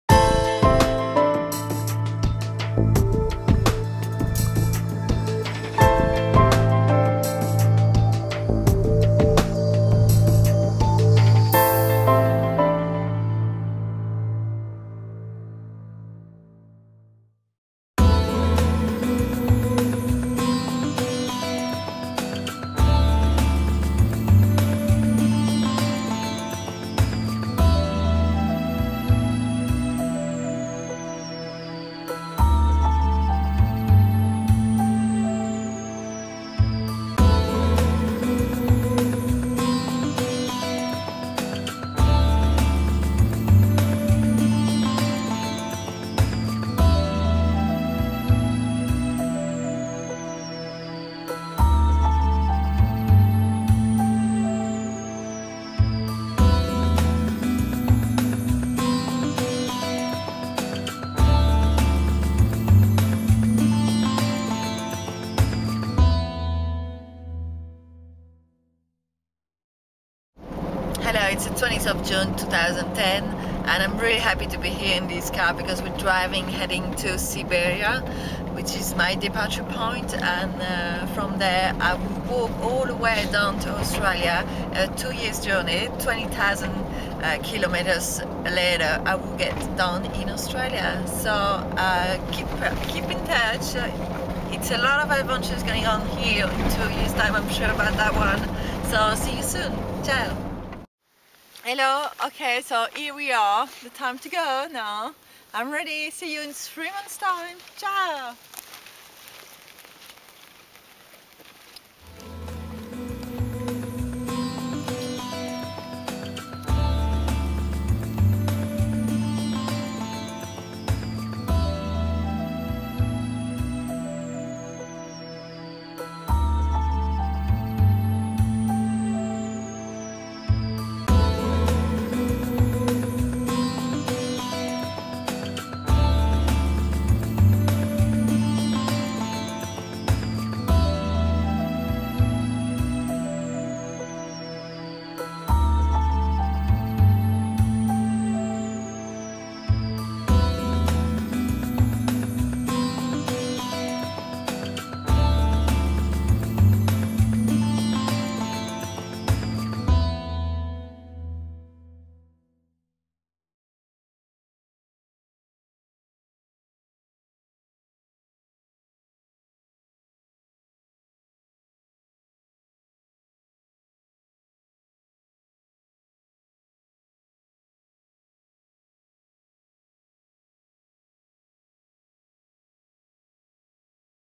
The Departure of Sarah Marquis June 20, 2010
Specification sheet : ExplorAsia

Departure: June 20th 2010 from Southern Siberia
Arrival: planned for June 20th 2012 in Australia
Kilometres: 20'000Km.

Estimated time (average): 2 years of hiking -- approximately 1000Km per month -- 30Km per day. Resting points: Total of 6 -- every 3 months -- a resting point is a place where Sarah will entirely renew her equipment and stop for a few days.